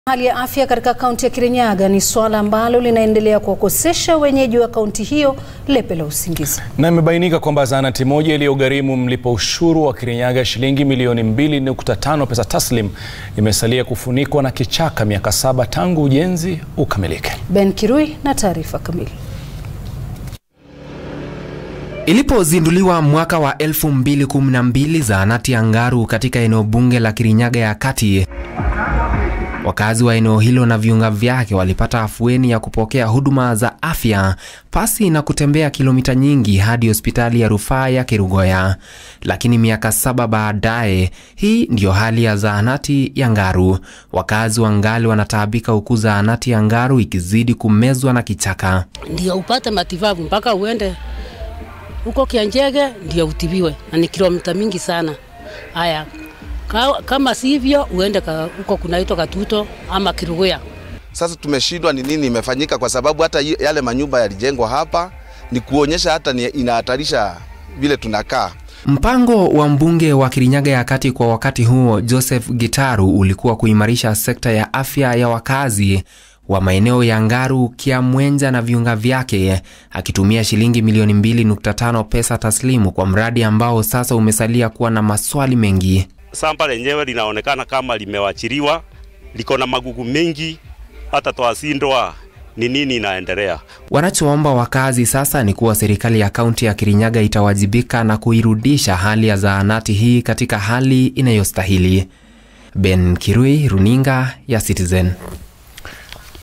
Zahanati iliyogharimu Ksh. 2.5 milioni yasalia kuwa kichaka
Hali ya afya katika kaunti ya Kirinyaga ni suala ambalo linaendelea kuwakosesha wenyeji wa kaunti hiyo lepe la usingizi. Imebainika kwamba zahanati moja iliyogharimu mlipa ushuru wa Kirinyaga shillingi millioni 2.5 pesa taslimu imesalia kufunikwa na kichaka miaka saba tangu ujenzi kukamilika.